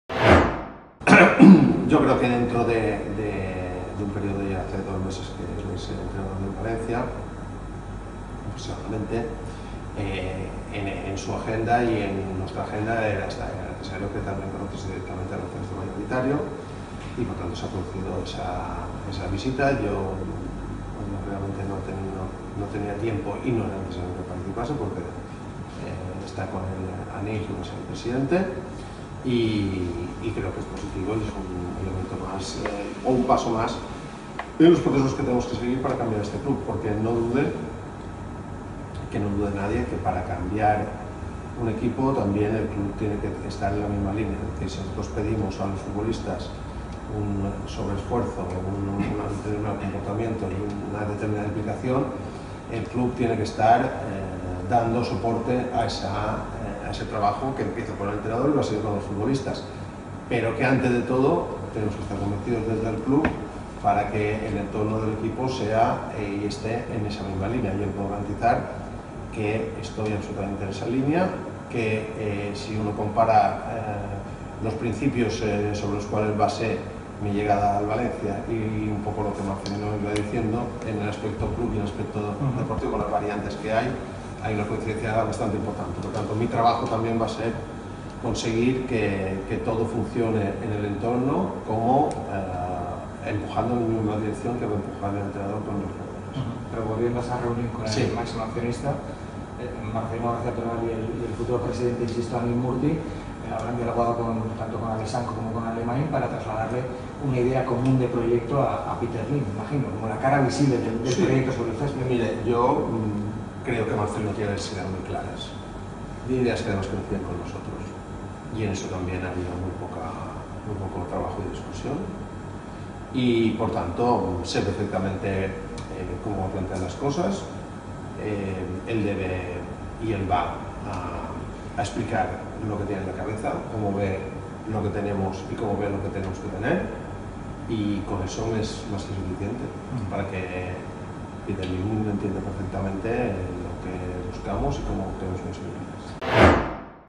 Alemany explica el encuentro entre Marcelino y Peter Lim en Singapur
Mateo Alemany ha explicado como será el encuentro entre Marcelino y Peter Lim en Singapur. Declaraciones en Cope.